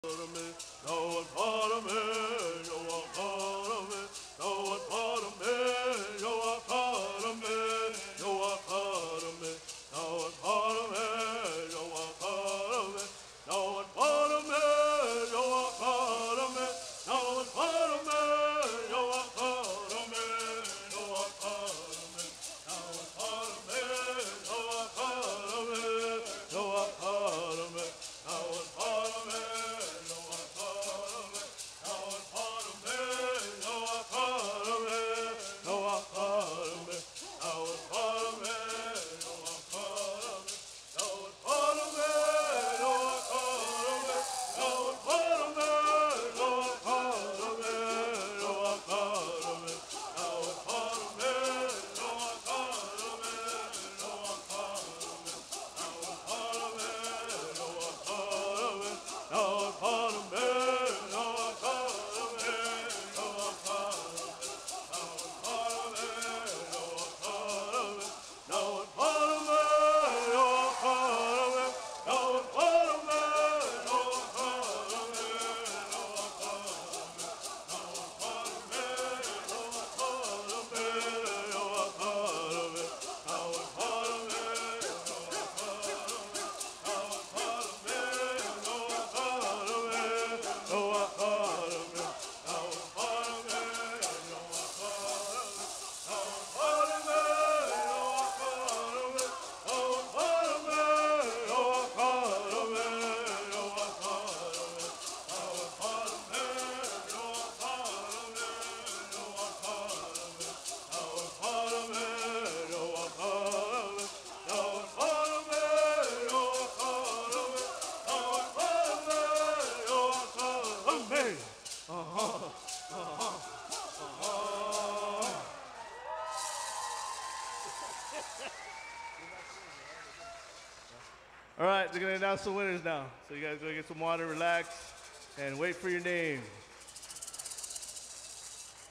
Bird Singing - 2021 Morongo Pow Wow - Powwows.com
Bird Singing
2021 Morongo Pow Wow
Cabazon, CA